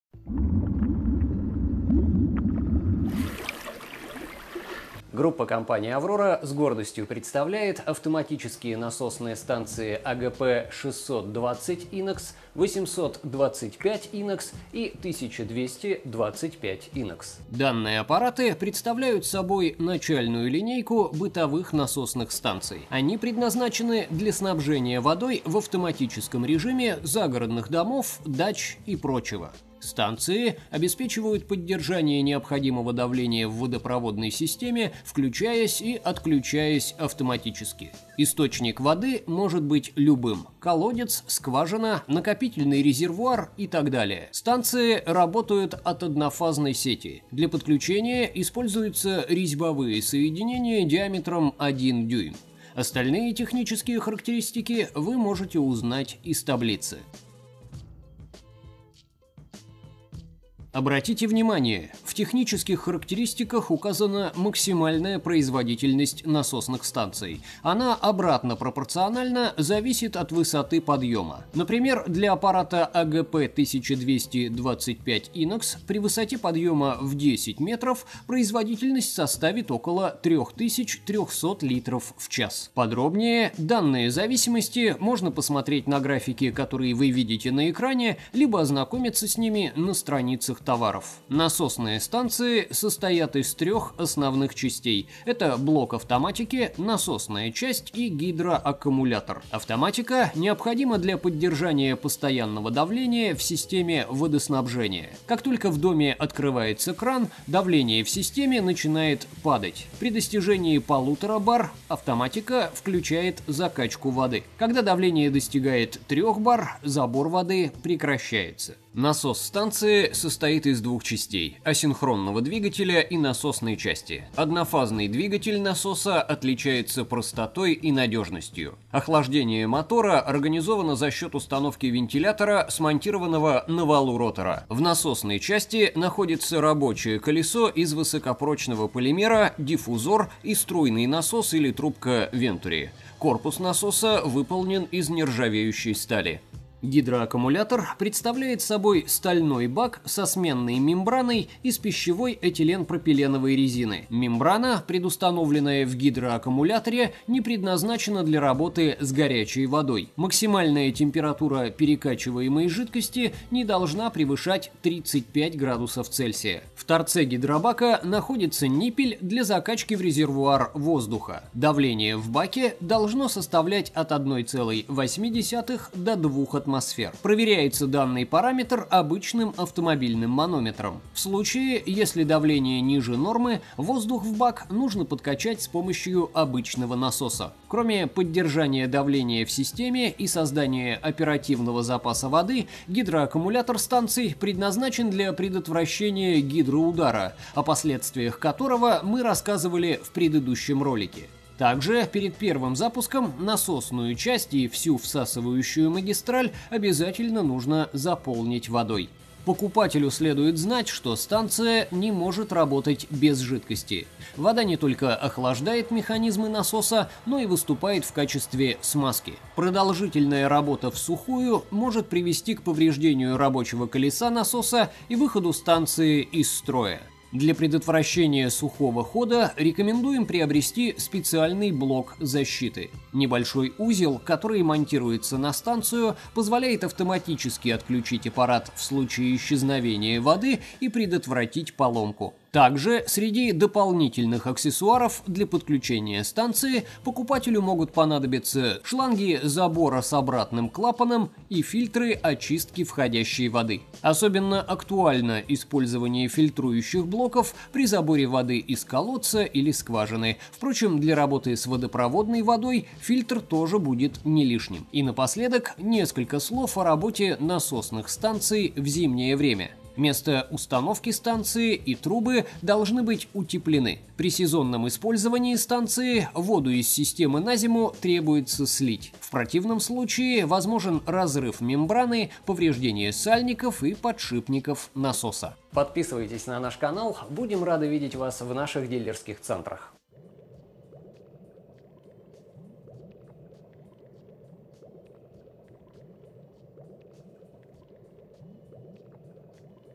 Обзор Линейка бытовых насосных станций AURORA
Перед вами линейка бытовых насосных станций AURORA: AURORA AGP 600-20 INOX, AURORA AGP 1200-25 INOX, AURORA AGP 800-25 INOX.

Атоматизированная высокопроизводительная насосная станция из нержавеющей стали,
работает тихо и надежно, способна поднимать воду с глубины до 8 метров.
Насосная станция имеет широкий спектр применения - от повышения давления в существующей системе водоснабжения
до полного обеспечения дома водой из скважин